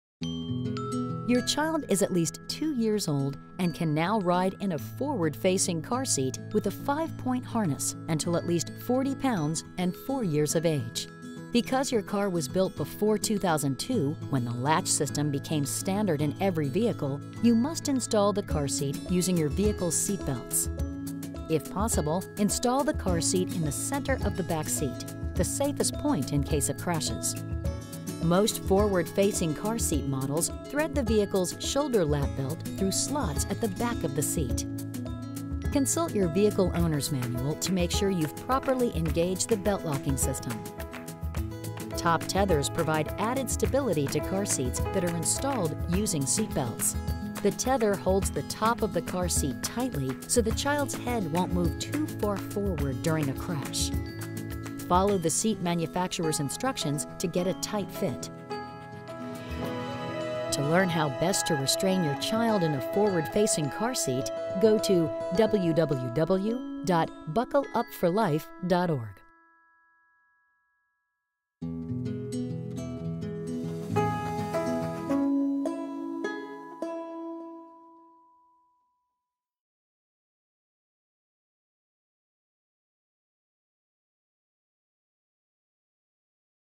Installing Forward-Facing Car Seat with Seat Belt
Tips on installing a Forward-Facing Car Seat using your vehicle's seat belts.

*Please refer to your car owner's manual and your car seat manufacturer's instructions for detailed information about properly installing your car seat in your particular vehicle. BuckleUpforLife.org